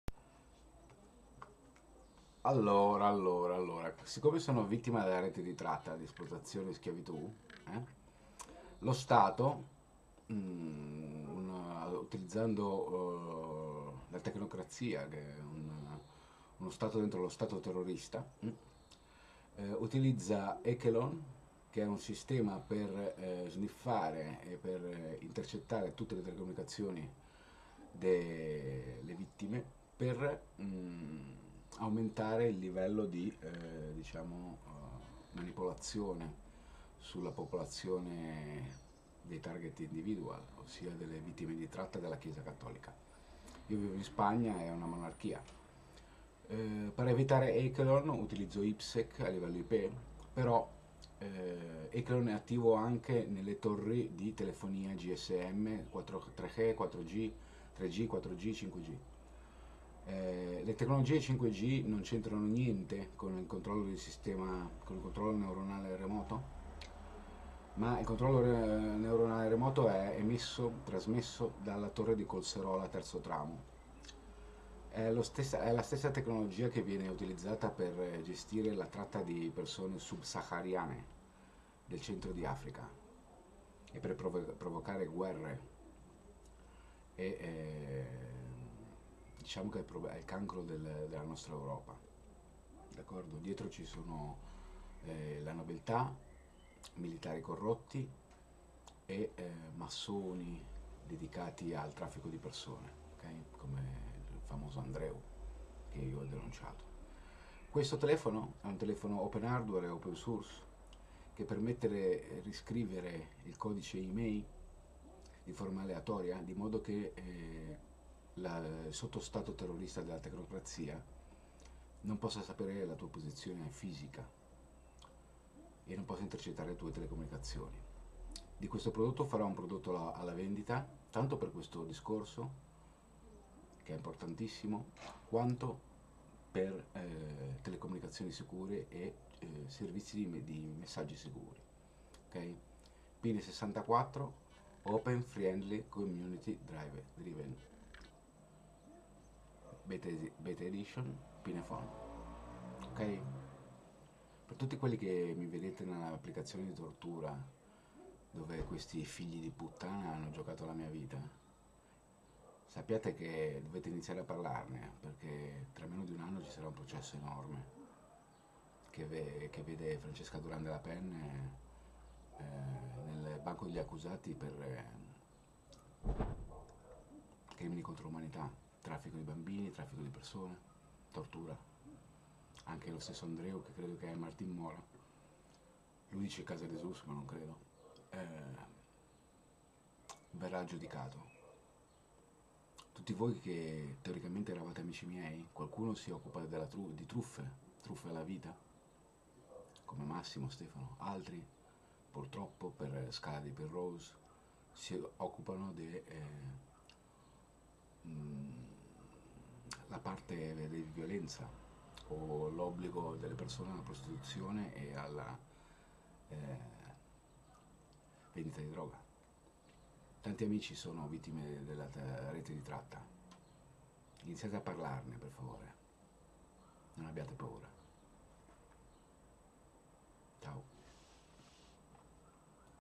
Pine64 phone to bypass echeleon in Spain
@thepine64 I've just received your phone!!! I'll use to bypass echelon used in Spain to manipulate and control the targetindividual community. People under neural control as you can appreciate above my head. Electronic weapons!